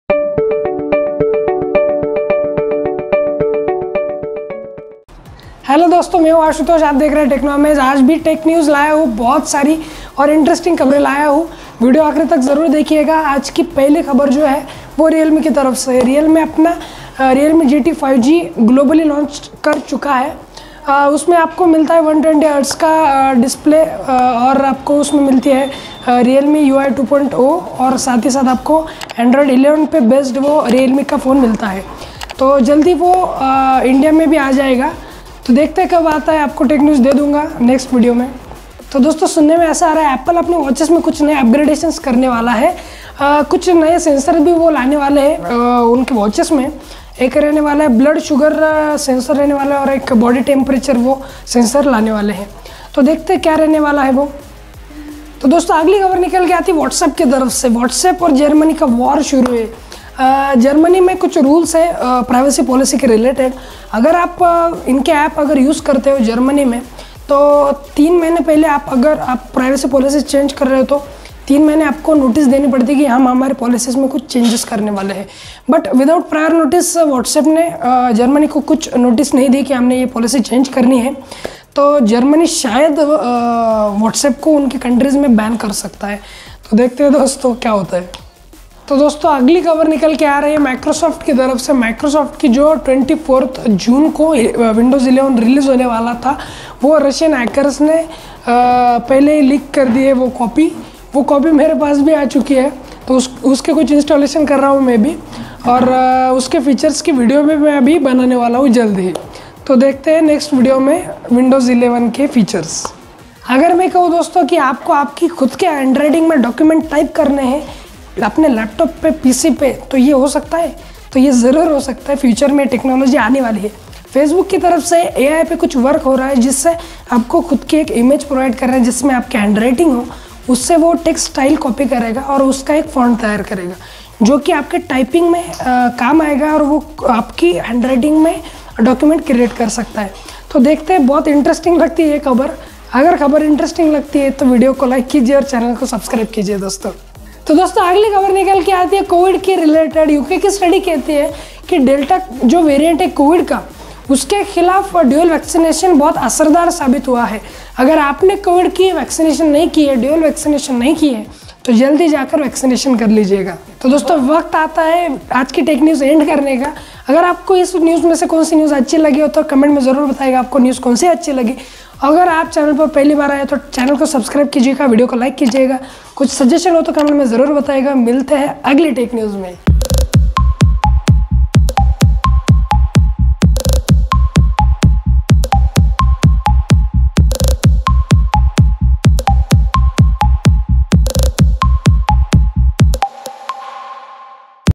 windows 11 leaked? Facebook will create your own handwriting | whatsapp Vs germany | technews ⚡⚡⚡
Tech News
1.Realme GT 5g Launched Globally
2.Apple Watch Series 7
3.In Germany they have declared war on WhatsApp
4.Windows 11 Really leaked?
5.Facebook Unveils AI Model That Copies Text Style From Images Using a Single Word
6.Two COVID-19 vaccine doses highly effective against Delta variant, finds UK study
time stamp
00:00 intro
00:12 Realme GT 5g
00:38 Apple watch
00:55 Germany Vs Whatsapp
01:28 windows 11 Leak
01:54 Facebook AI
02:28 Covid 19 vaccine